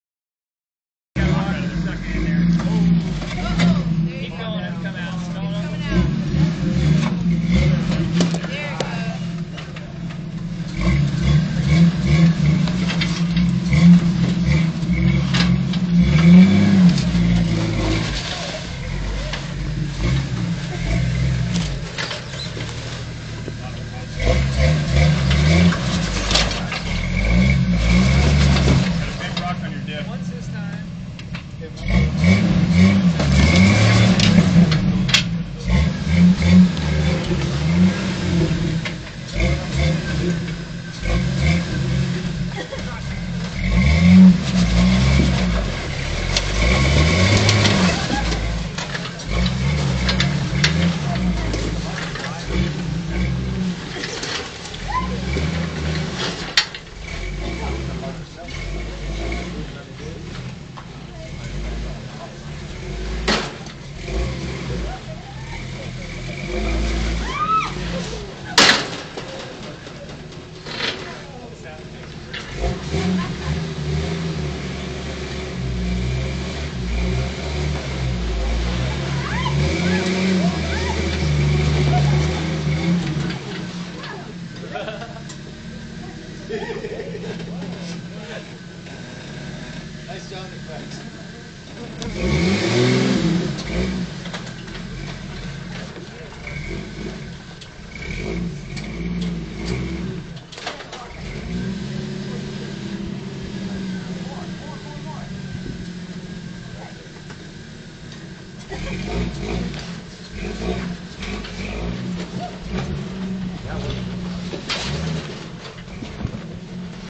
Keith & Lynn's Jeep YJ Rock Crawling @ Rock Bottom Logandale NV
Keith is pre-running the Rock Bottom Trail for the 2012 Hump n Bump that is taking place the first weekend of November.
Jeep Wrangler Accessories
Jeep Wrangler Armor